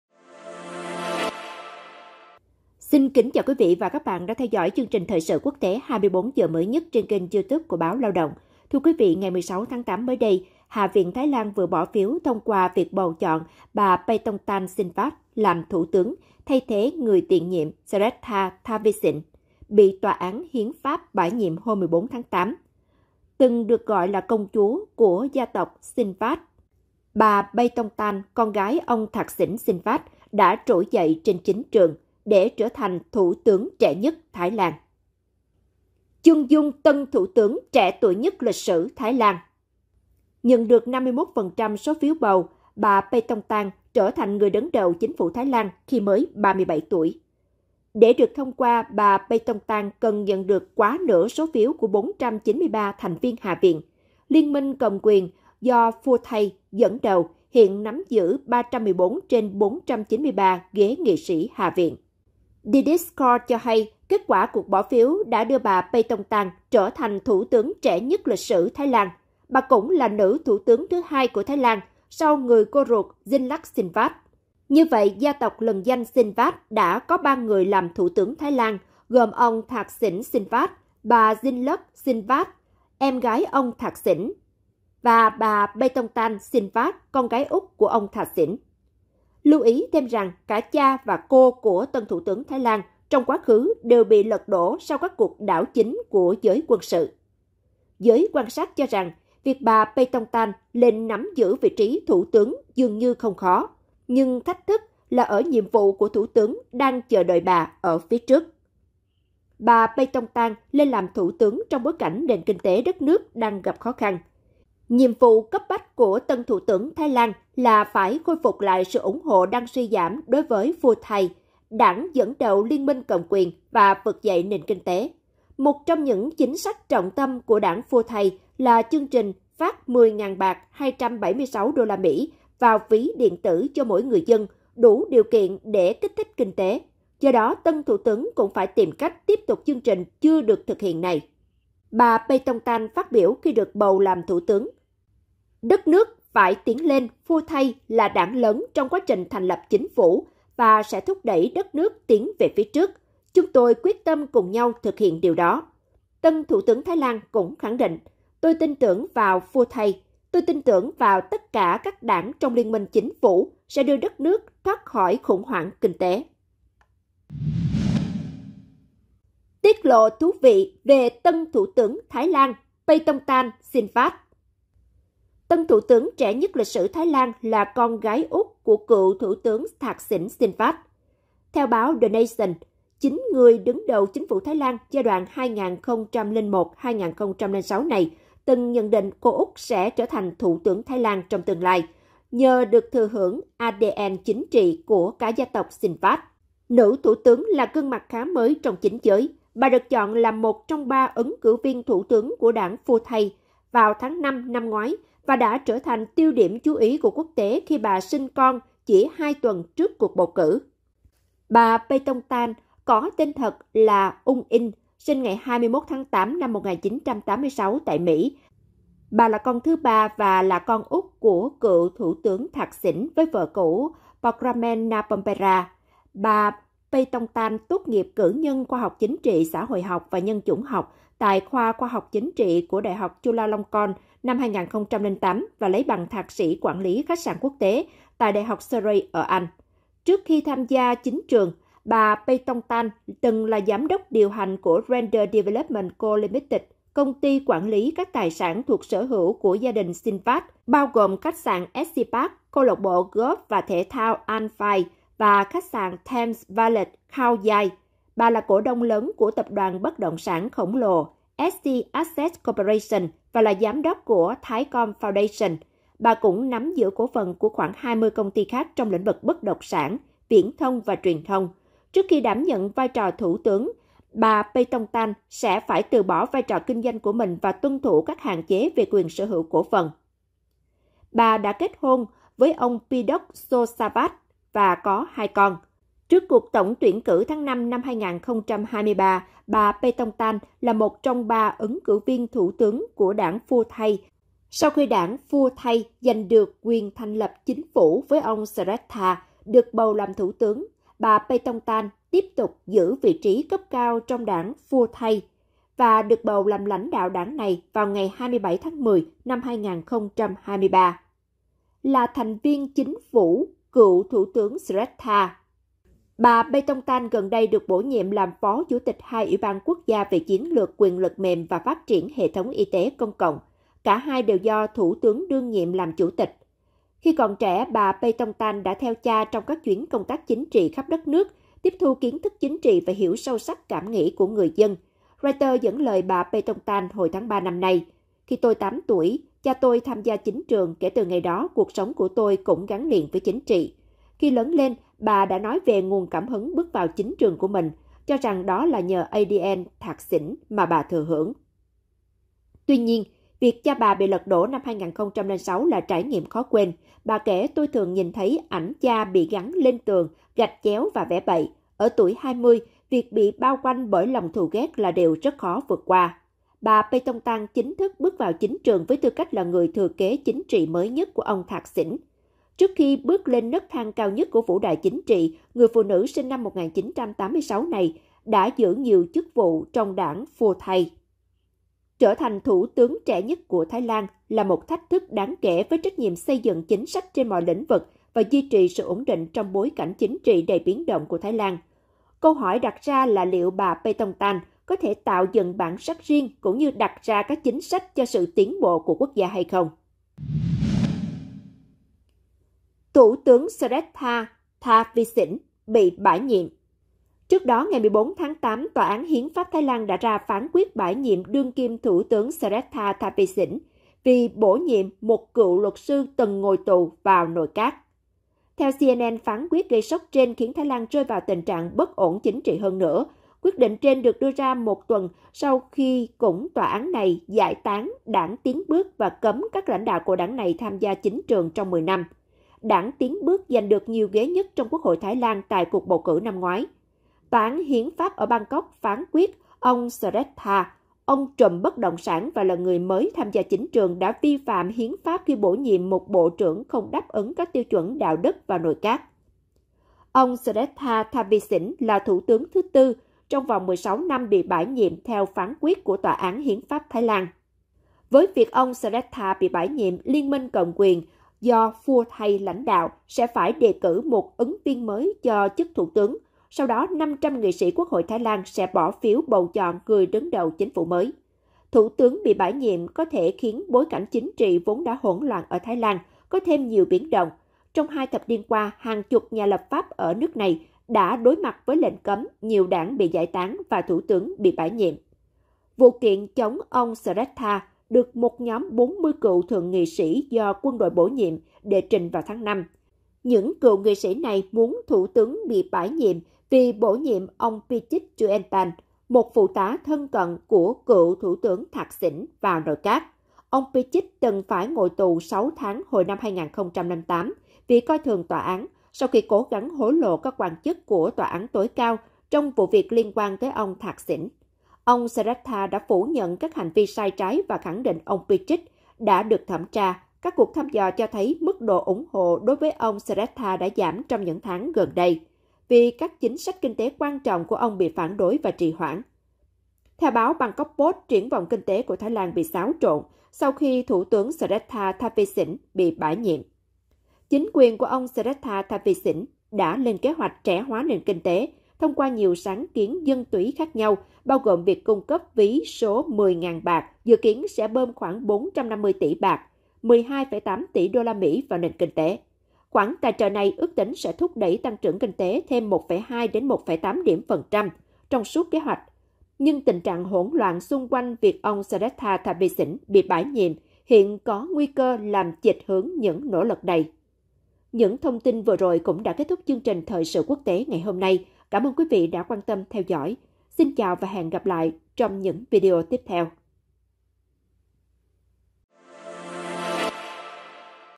Chân dung tân thủ tướng trẻ tuổi nhất lịch sử Thái Lan: Người kế thừa số phận chính trị của gia tộc
BÁO LAO ĐỘNG | Ngày 16/8 mới đây, Hạ viện Thái Lan vừa bỏ phiếu thông qua việc bầu chọn bà Paetongtarn Shinawatra làm thủ tướng, thay thế người tiền nhiệm Srettha Thavisin bị Tòa án Hiến pháp bãi nhiệm hôm 14-8. Từng được gọi là “công chúa” của gia tộc Shinawatra, bà Paetongtarn, con gái ông Thaksin Shinawatra, đã trỗi dậy trên chính trường để trở thành thủ tướng trẻ nhất Thái Lan.

* Đường dây nóng: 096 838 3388